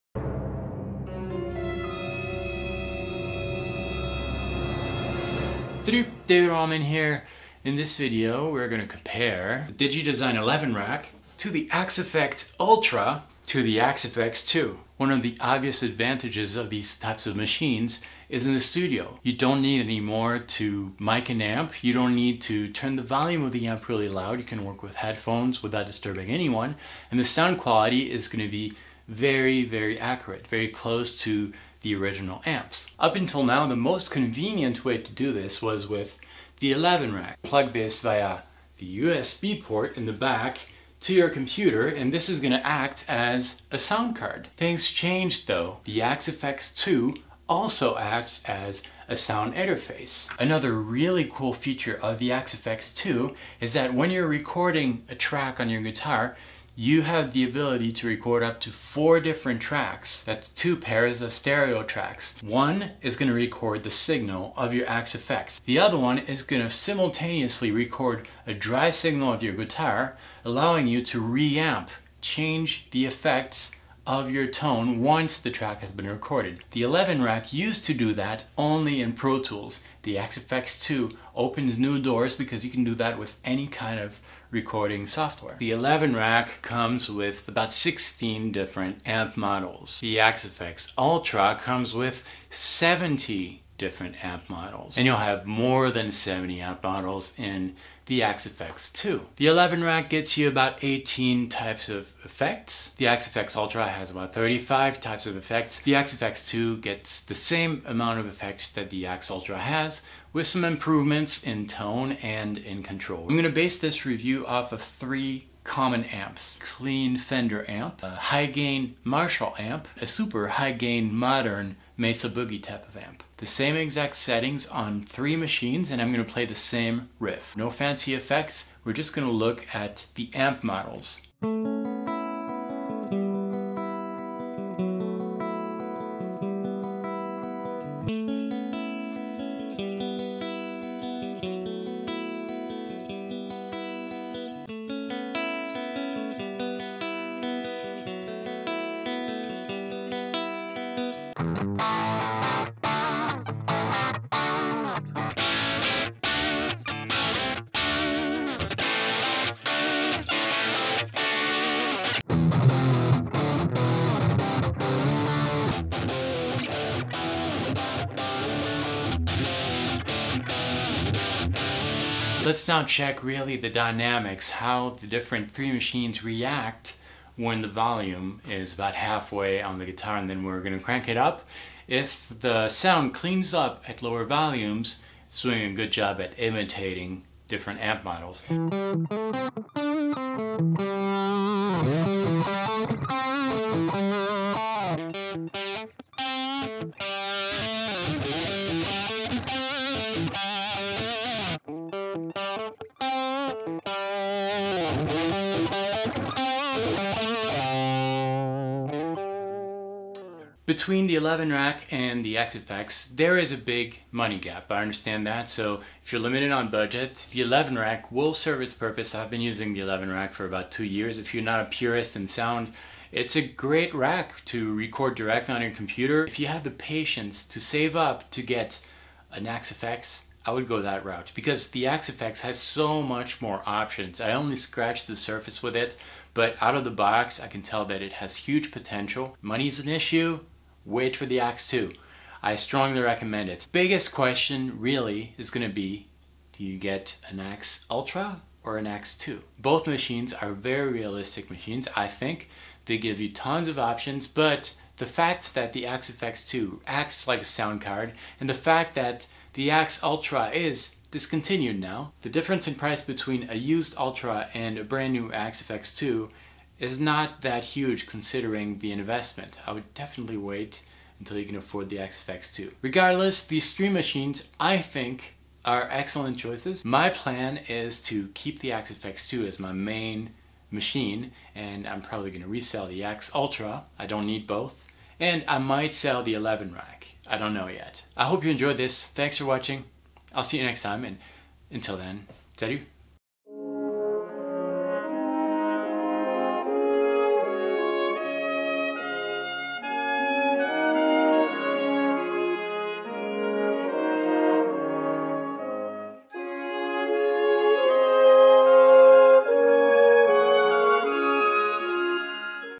Axe FX Ultra vs Axe FX II vs Eleven Rack
This video compares three amp models found in Fractal Audio's Axe FX Ultra, Axe FX II and Digidesign Eleven Rack.
The amp models used are based on a Fender Deluxe 64, Marshall JCM 800 and Mesa Boogie Dual Rectifier. No effects were added so that you can really compare the amps.

Guitar used: Parker Nitefly